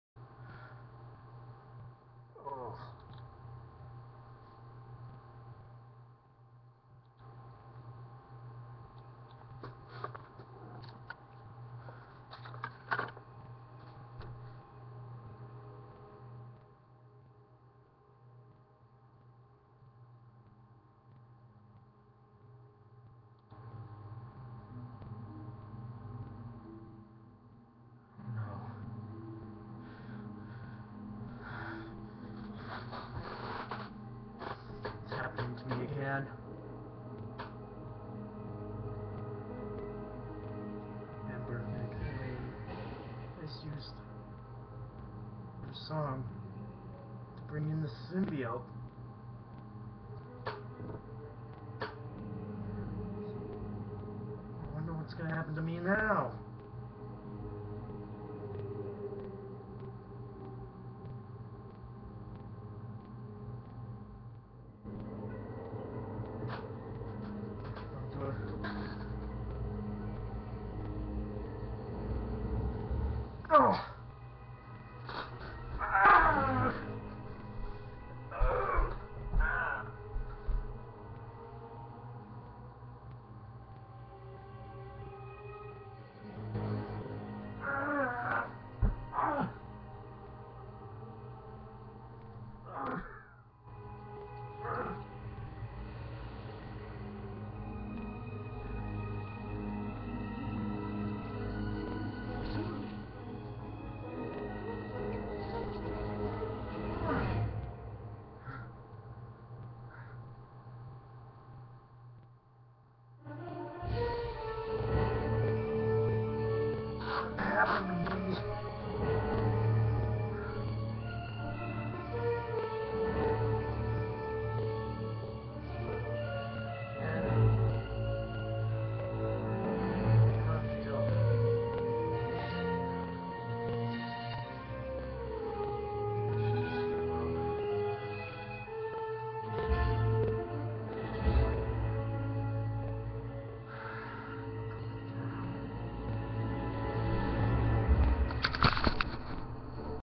Transformation To The Black Suit Spider-man (Joe Isaacson)
This is not good guys because Ember McLain(from Danny Phantom) Use Her Song to put a curse on me.